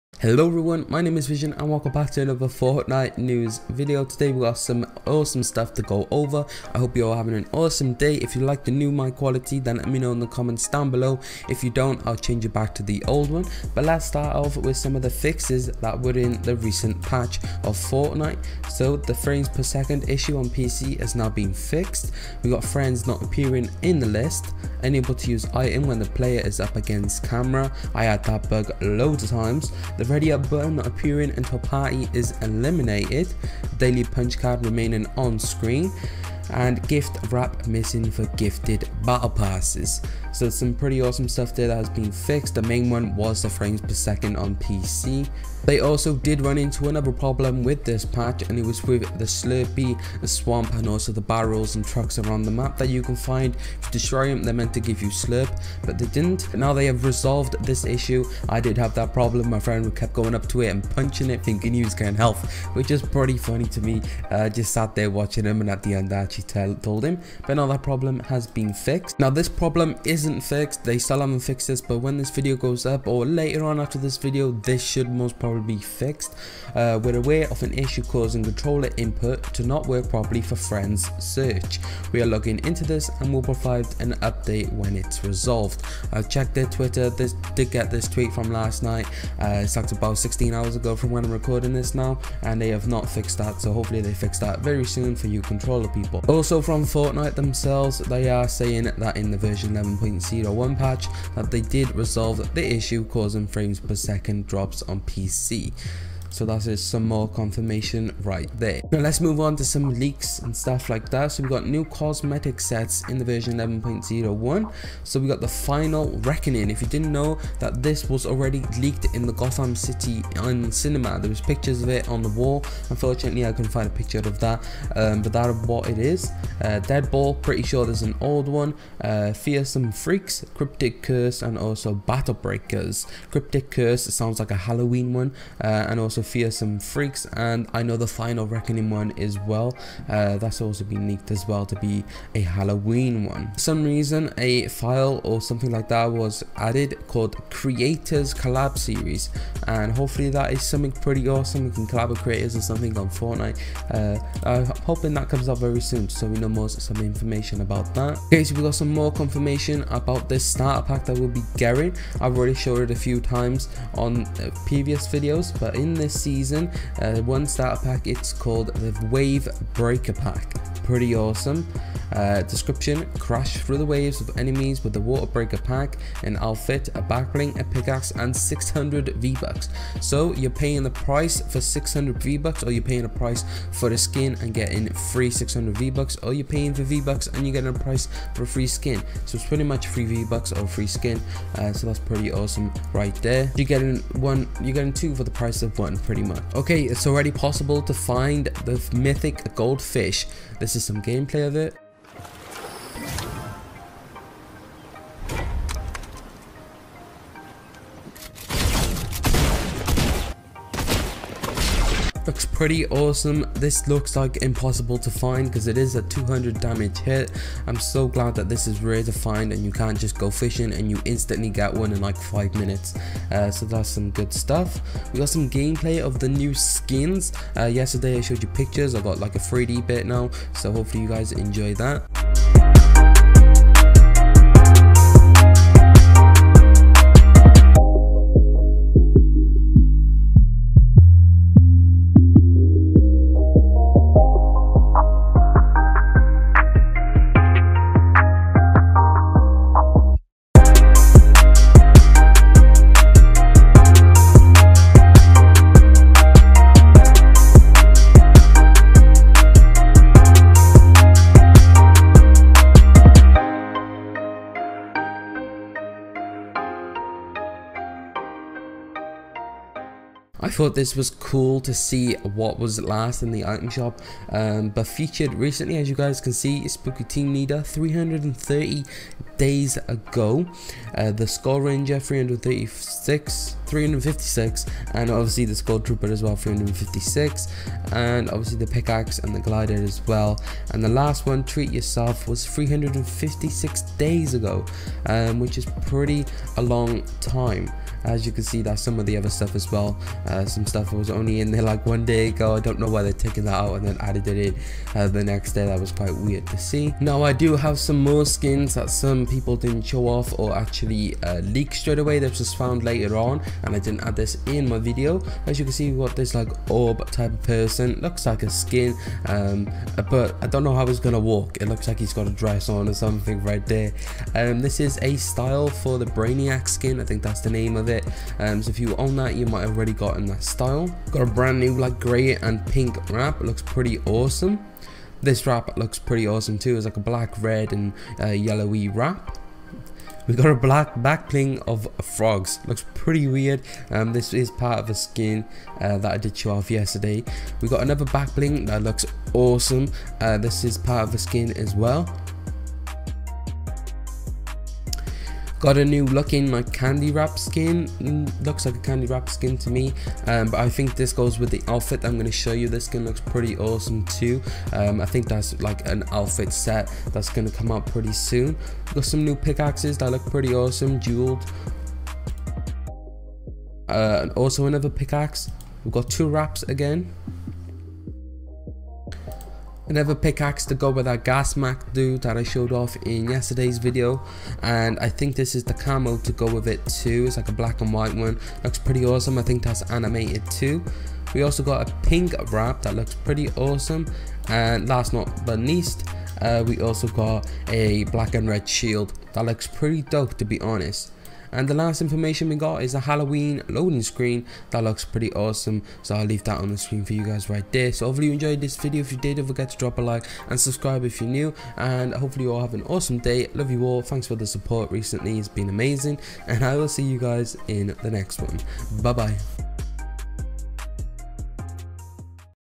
*NEW* Fortnite Update | NEW LEAKED BUNDLES | FREE REWARDS | Halloween Update | v11.01 News
Today we discuss the new leaked bundles and also SOME new skins that were leaked

USE CODE IN ITEM SHOP - Vznxx

Music IS NOT owned by me they are free royalty music 😀

Artist
Enyer Gonzalez

Vizion Vznxx Vzxx